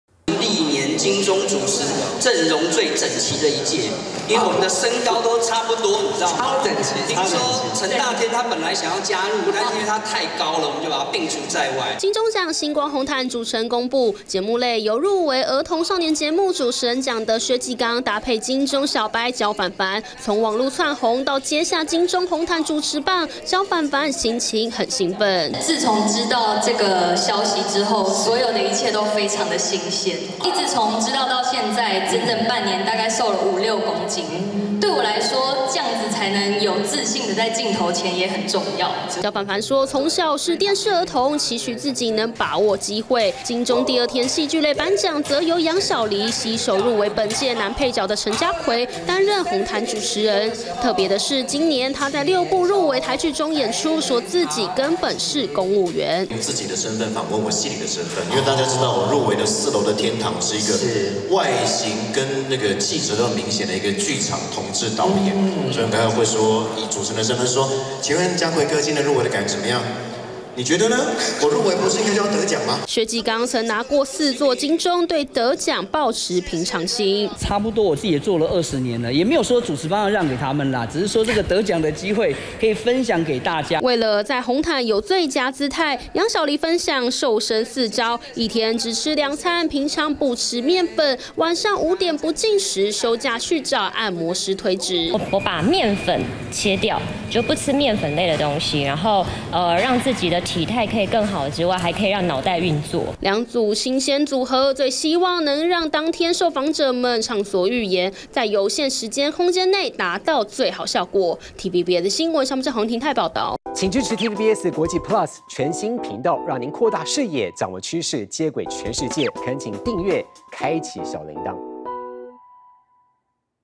金鐘紅毯主持！「節目」薛紀綱+焦凡凡 「戲劇」楊小黎+陳家逵｜TVBS新聞
本屆金鐘獎即將在下個月21.22號登場，星光大道主持人也出爐，由薛紀綱跟焦凡凡主持第一天的綜藝類紅毯，楊小黎跟陳家逵則主持戲劇類，新鮮組合讓觀眾眼前為之一亮。